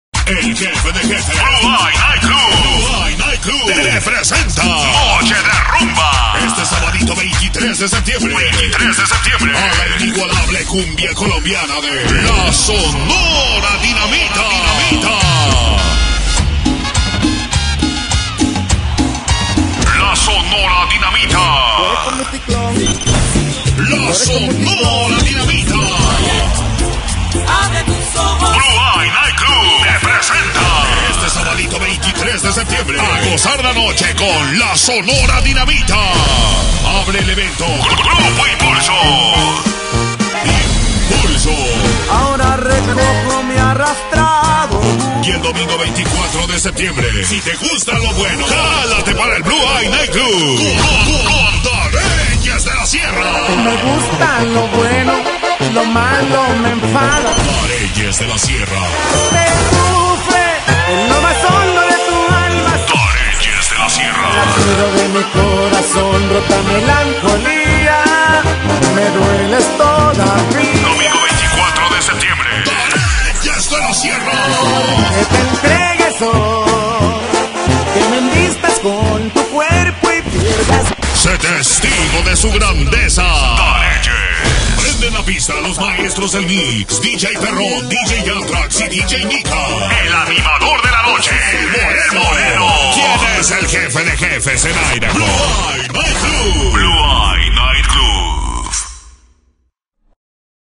Sabado 23 La Sonora Dinamita Domingo 24 Los Dareyes De La Sierra
No te puedes perder este fin de semana solo en el Blue Eye Nightclub!
Sábado 23 de Noviembre se presenta La Sonora Dinamita y abriendo el baile Grupo Impulso!

Y para cerrar la semana con broche de oro este Domingo 24 de Septiembre se presentan Los Dareyes De La Sierra!